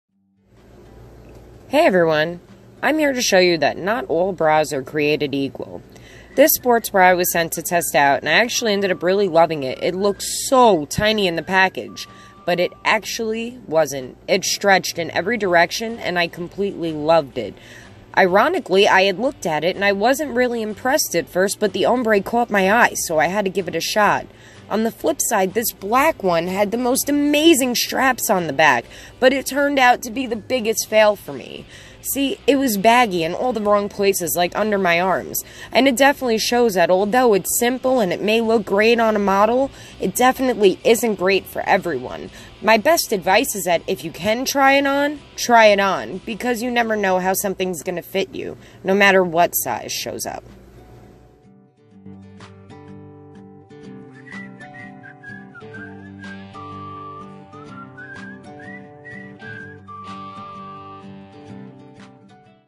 Never Judge a Book by its Cover
Hey there friends, I just wanted to say something to you all. When you're shopping, if something looks good but you're unsure of it - try it on. I was surprised with the outcome of my product tests and it was very eye opening.

The cute bra didn't fit right at all, yet the practical one fit great. The grey one looked so small but it was perfect, whereas the black looked perfect and was baggy under my arms.